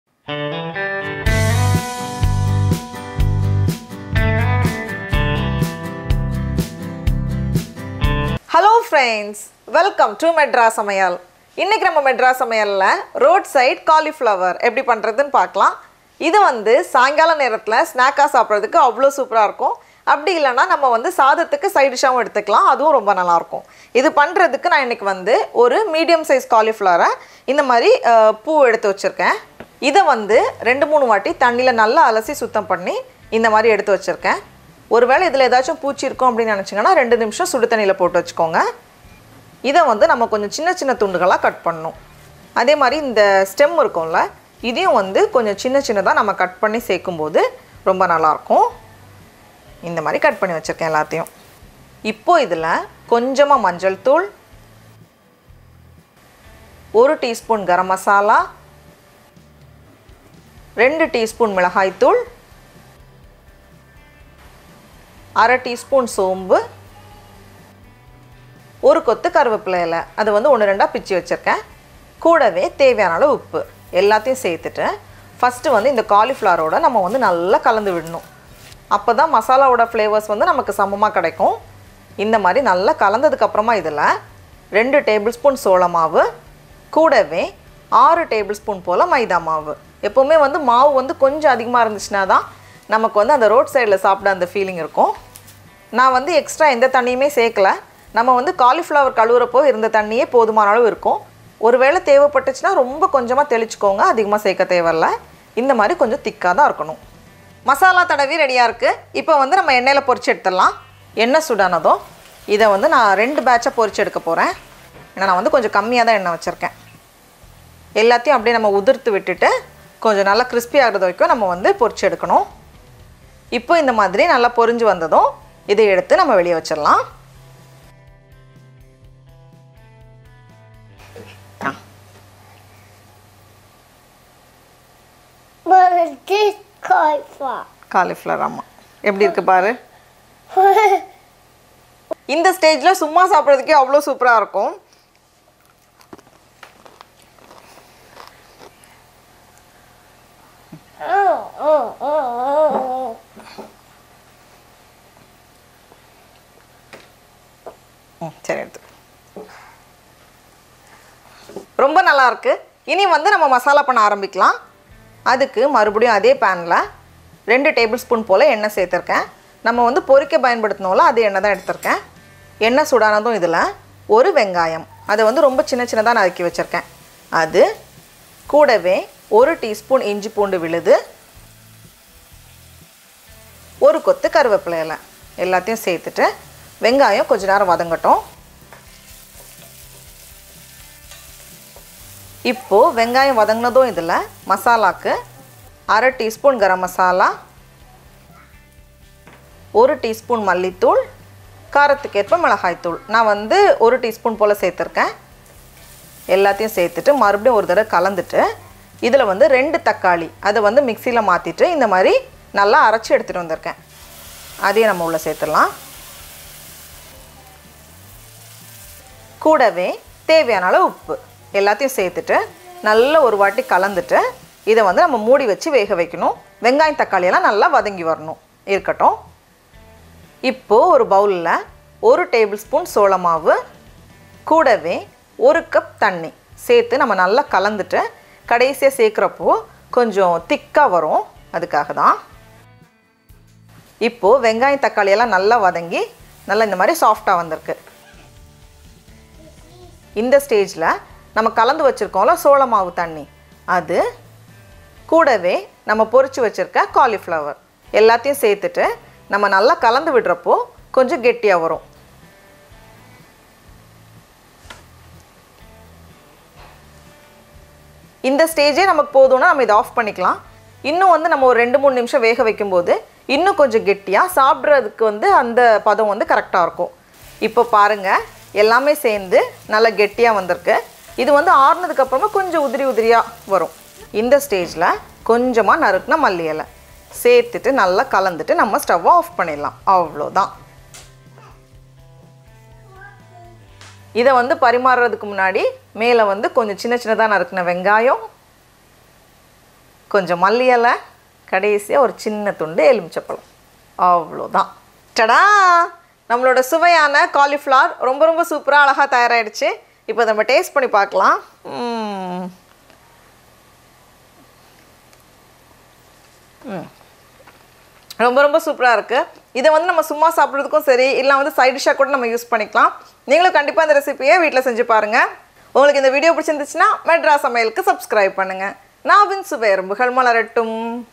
Roadside Cauliflower Recipe in Tamil | Streetstyle Cauliflower Masala in Tamil | Gobi Masala
Ingredients:
1 Cauliflower (350 g)
1/4 tsp turmeric powder
1 tsp garam masala
2 tsp Chili powder
1/2 tsp Fennel seeds
Curry leaves
required Salt
2 tbsp Corn flour
6 tbsp Maida
Oil for frying
2 tbsp Oil
1 Onion
1 tsp Ginger garlic paste
Curry leaves
1/2 tsp Garam masala
1 tsp Coriander powder
1  tsp Chili powder
2 Tomatoes
1 tbsp Corn flour+1 cup Water
Coriander leaves